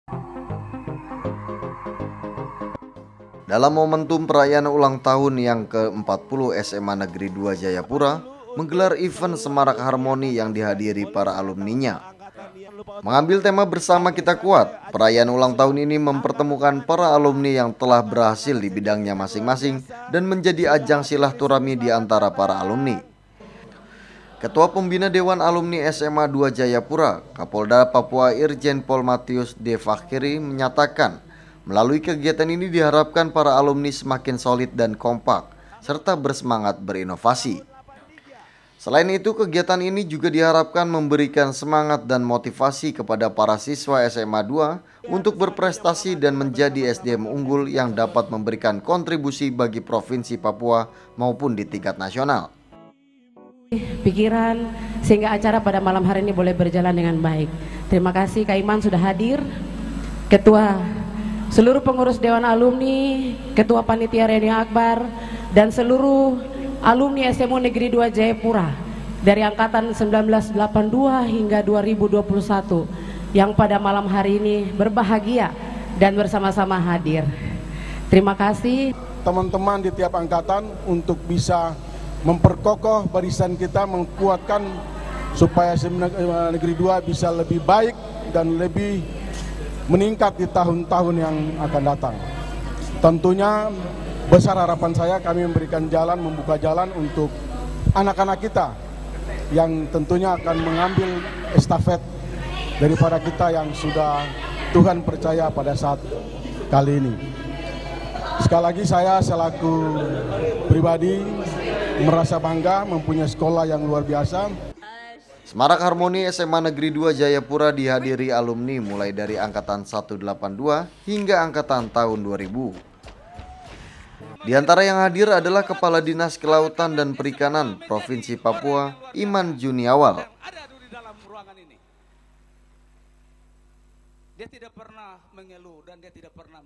KONTRIBUSI SMA N 2 JAYAPURA DALAM PEMBANGUNAN SDM DI PAPUA
PARA ALUMNI SMA NEGERI 2 JAYAPURA BERKOMITMEN UNTUK TERUS BERKONTRIBUSI DALAM PEMBANGUNAN SDM MUMPUNI DI PAPUA//
HINGGA KINI SMAN 2 JAYAPURA TELAH MENGHASILKAN LULUSAN BERPRESTASI DI BERBAGAI SEKTOR YANG TELAH BERKIPRAH DI TANAH PAPUA DAN TINGKAT NASIONAL///